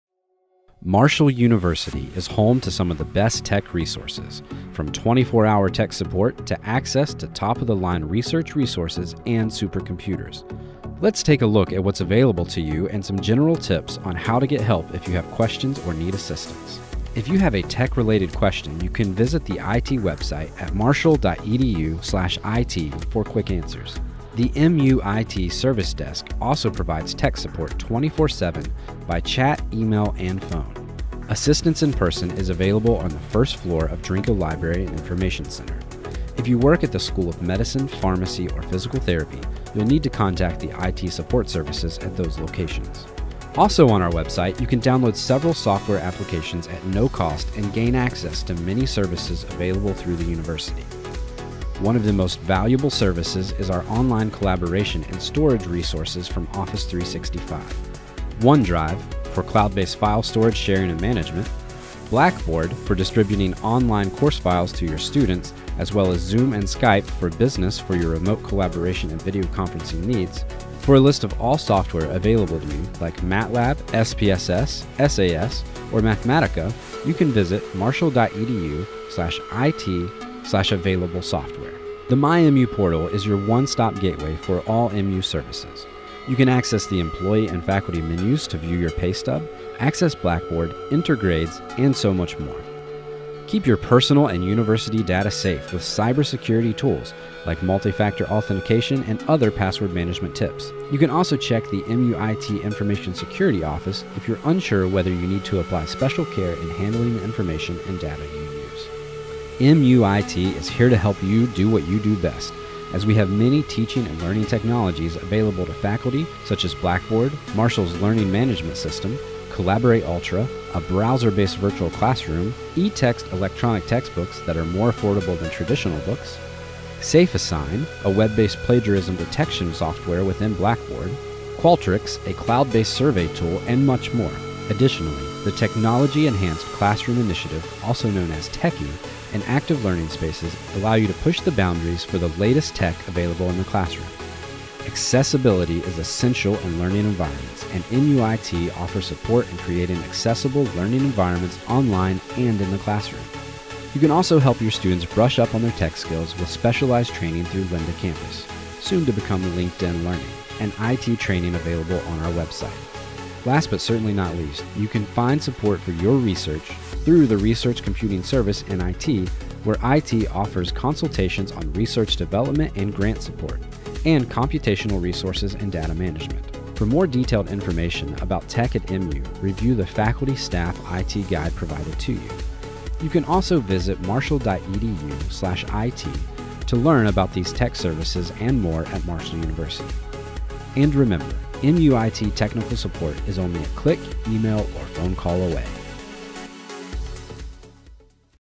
Faculty and Staff IT Resources at Marshall University
Watch this short video and learn all about the Faculty and Staff IT Resources at Marshall University.

-Contact the IT Service Desk-